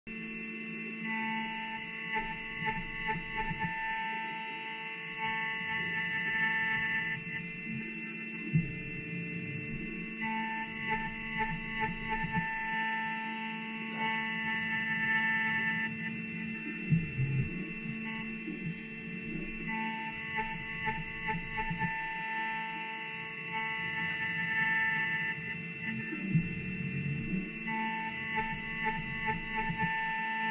Kabuto Finishing Pass
The Blaser technology center making a finishing pass on our Kabuto project.